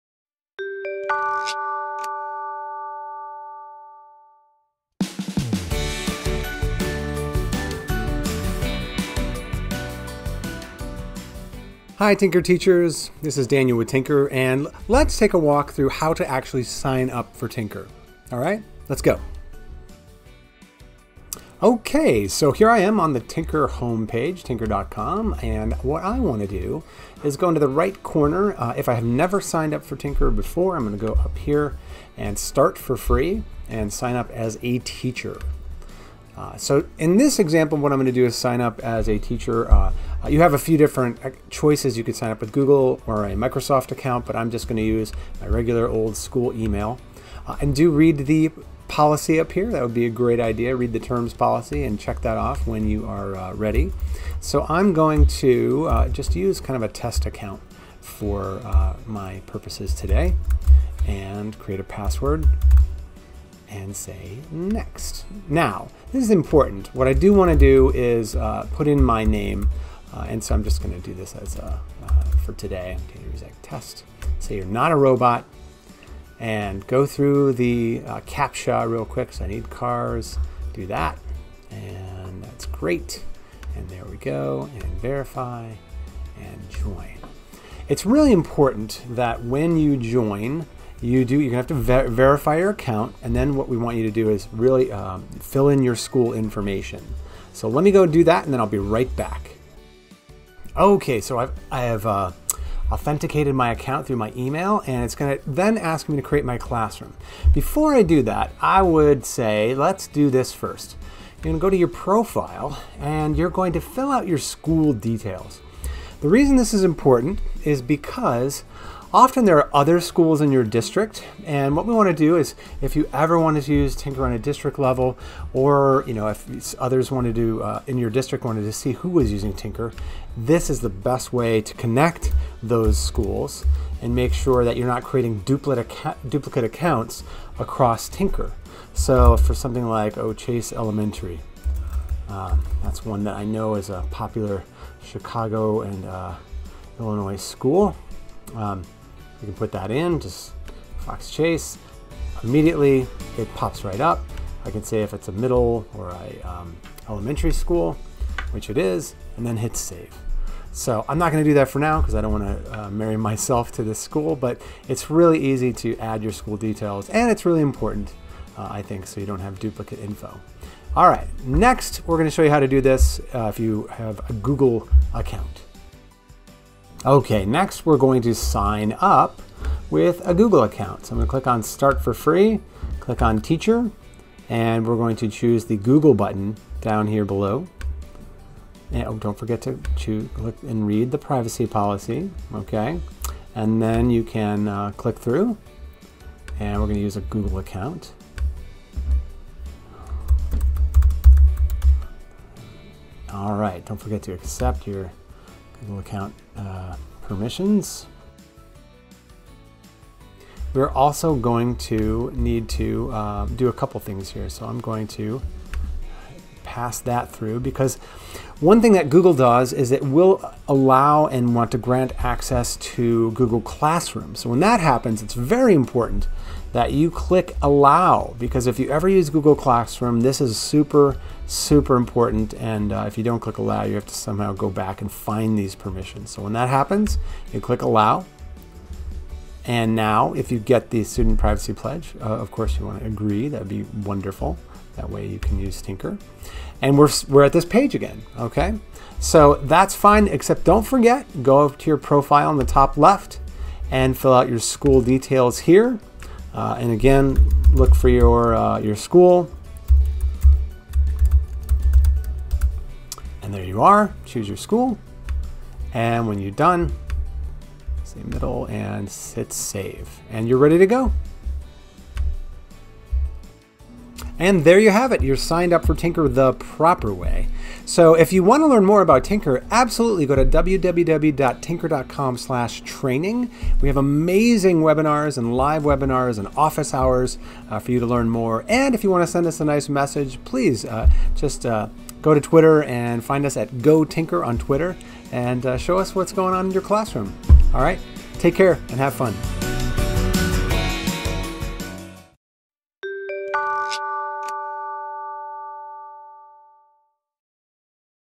Signing up for Tynker with Google and Email (for Teachers)
#1 thing you want to remember when signing up as a teacher is to fill out your school information! This ensures that any other teachers that are in your school or district are under the same umbrella, and it prevents creating duplicate schools.